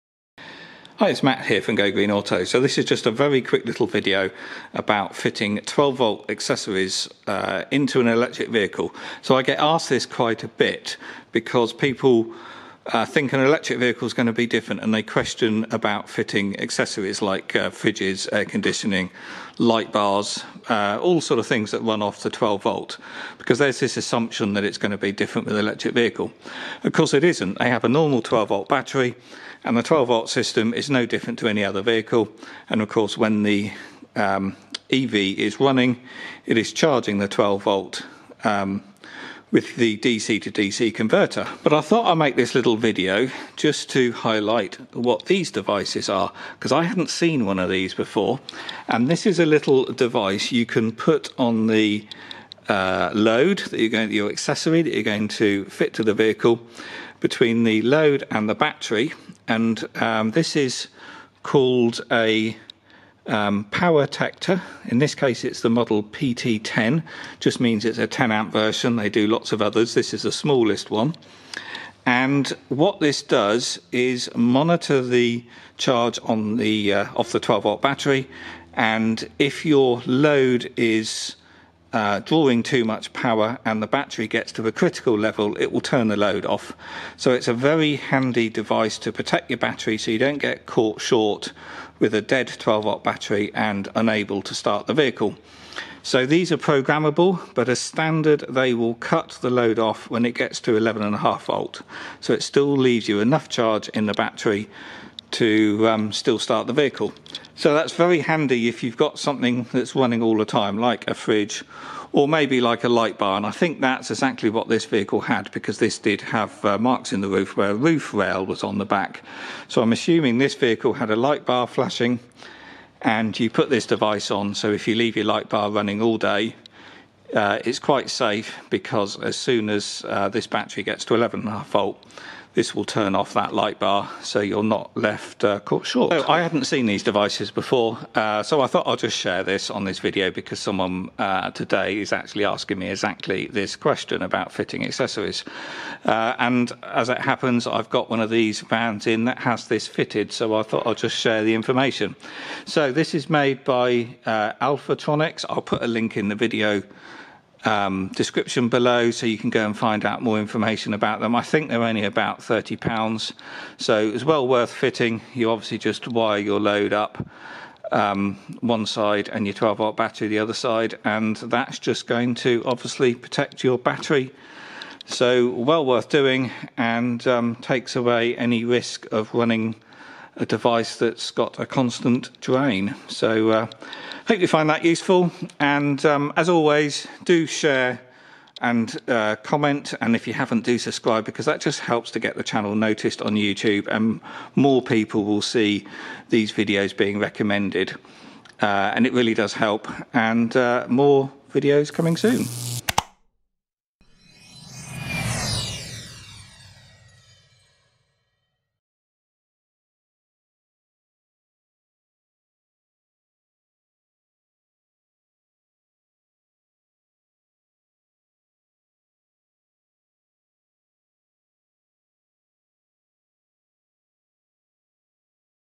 Fitting 12V accessories in an Electric Vehicle (& any other vehicle too)
In this video I quickly answer a question I get asked many times in regard to fitting 12V accessories to an electric vehicle. A Renault Kangoo van currently in stock has a Alfratronix PowerTector solid state battery guard fitted, which is a device I knew nothing about, but looks a good idea to protect your 12V battery from draining. You can find about more about these devices here

This video was made using WeVideo, the online cloud-based video editing software.